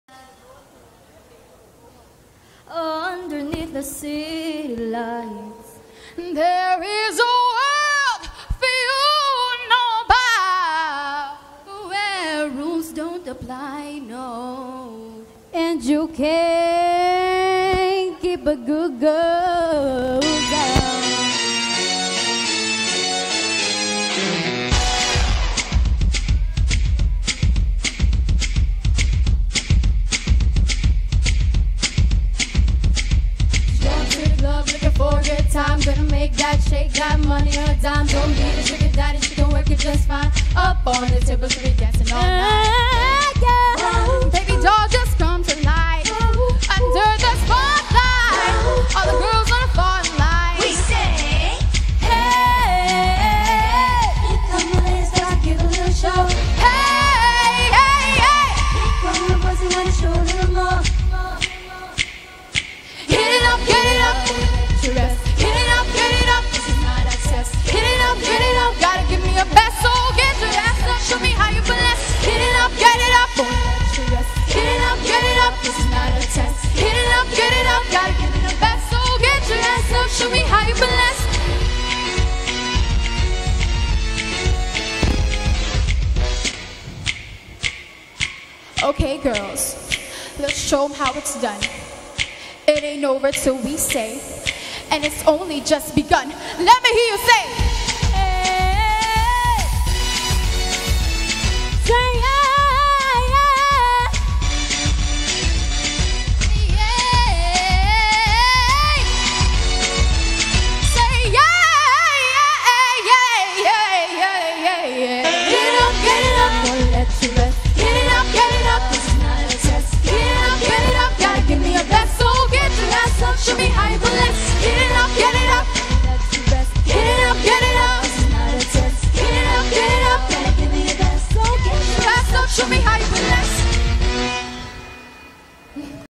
Cebu Music Learning Center "Tribute To Legends" Prod
Summer 2016 Concert "Tribute to Legends"
Our students Marjhan, Erika, Rachel, Christine on their performance @ Ayala Terraces, May 29, 2016.